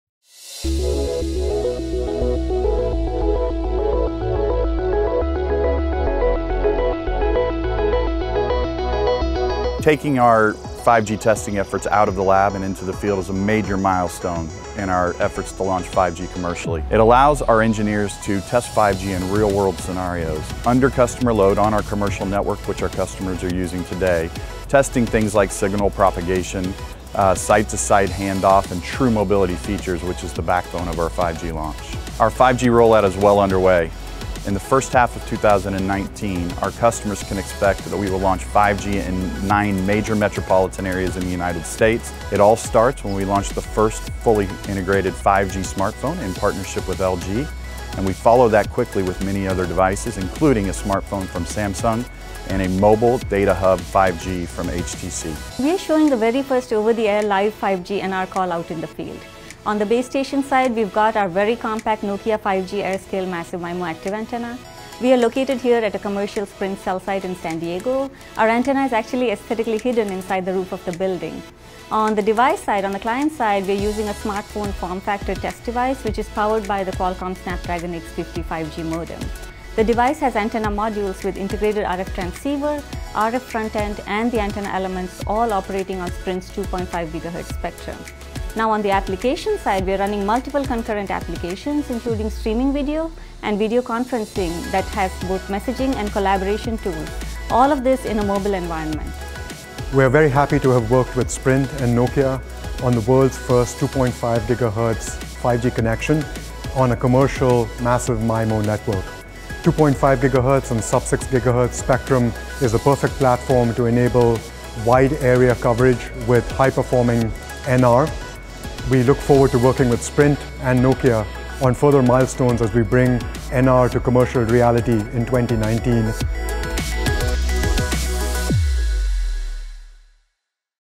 First 5G call with 2.5GHz and mMIMO - Sprint, Qualcomm and Nokia
Watch the world’s first 5G data call using 2.5GHz and massive MIMO on Sprint’s live commercial network.